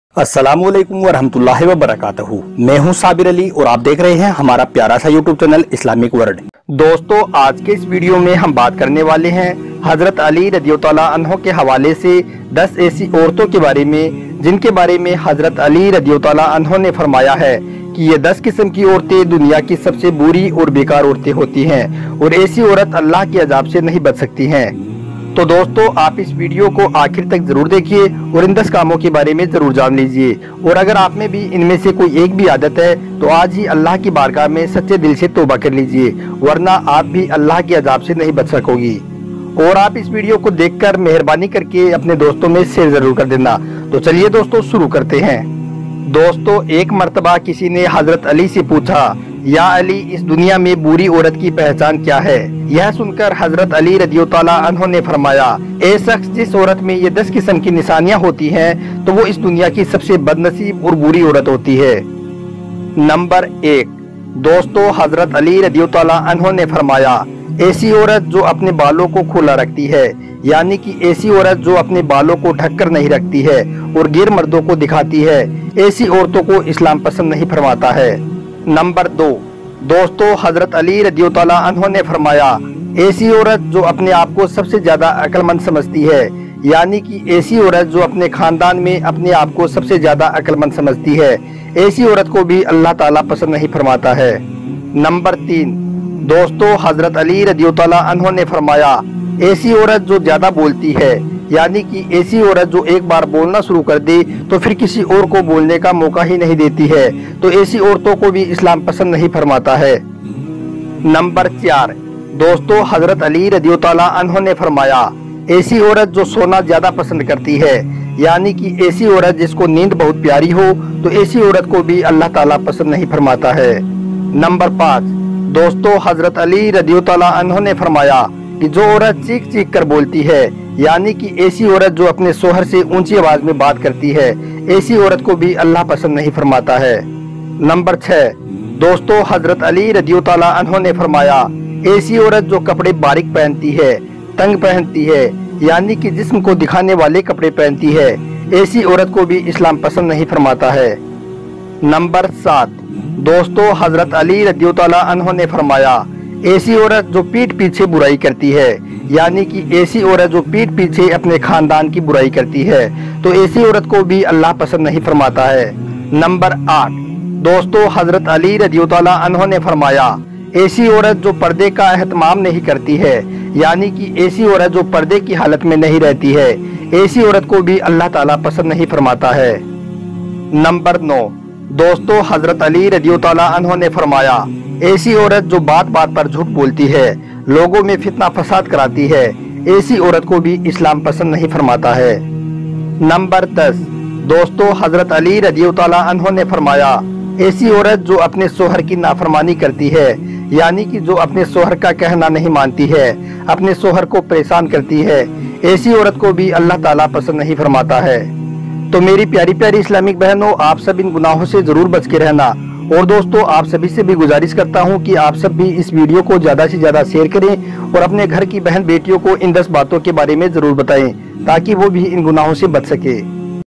Hazrat Ali (R.A) Ne Farmaya - इस्लाम में बुरी और बेकार औरत की 10 निशानियां
aaj ke is video me aapko hazrat ali ke hawale se bataya gaya hai 10 esi aurato ke bare me jinko allah paak pasand nahi farmata hai yani ye 10 aurate islam me bahut hi buri or bekar aurate hajrat ali ne batai ha to dosto aap is video ko aakhir tak dekhiye or apne dosto me share bhi kar dijiye shukriya apka